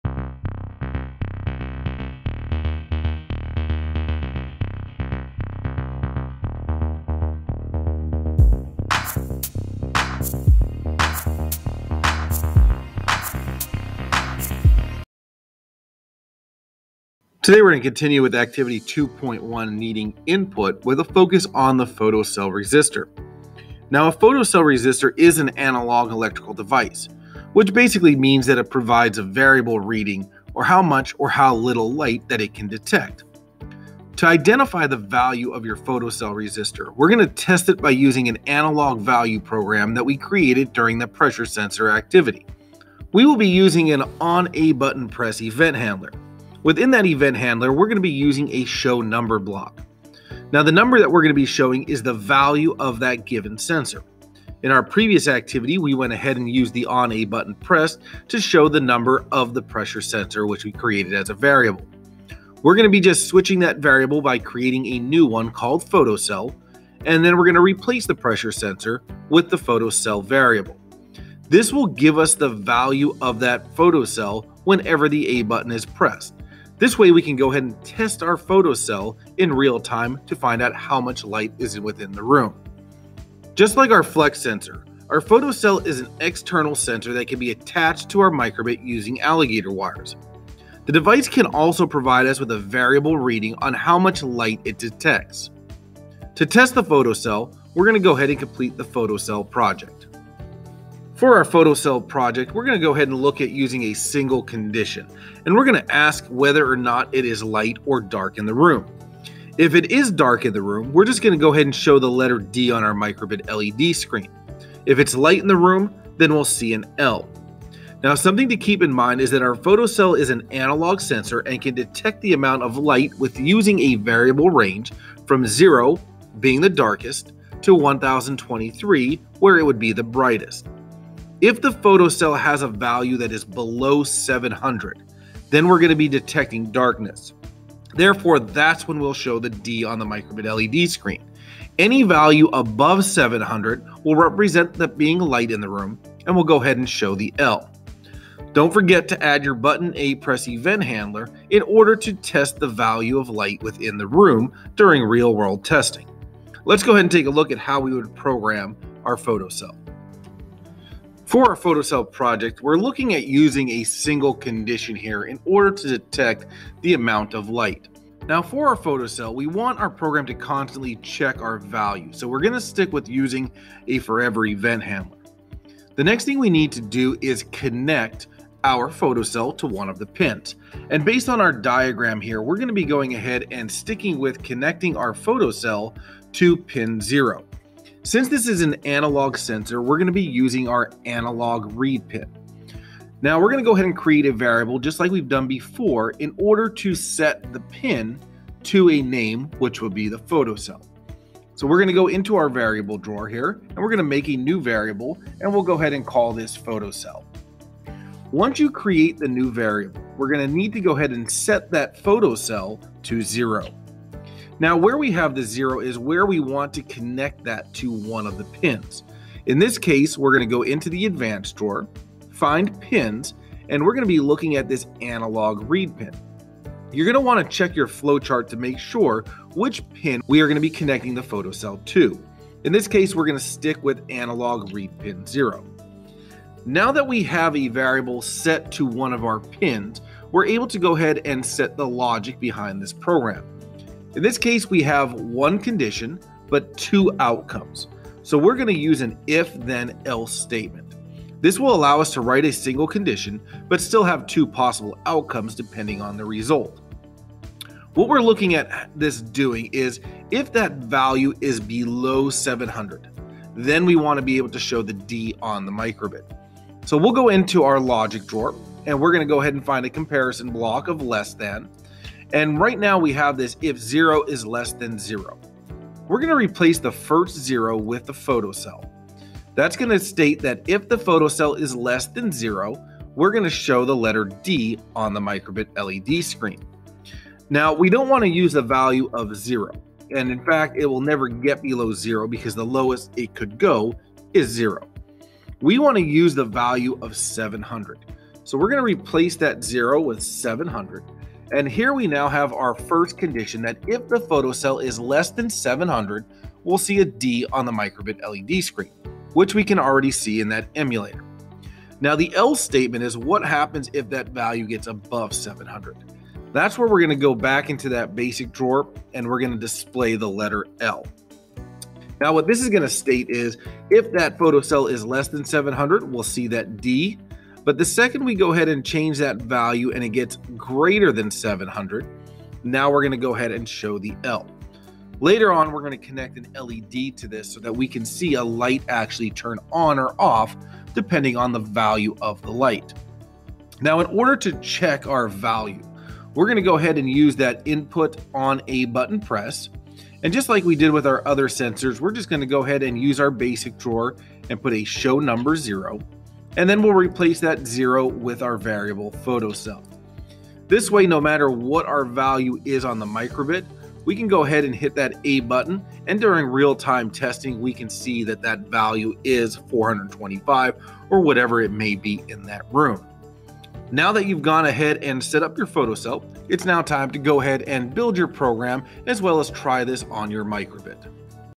Activity 2.1 - Using a Photocell with your Micro:bit
In this video, I walk through the various steps required to use a Photocell with your Micro:bit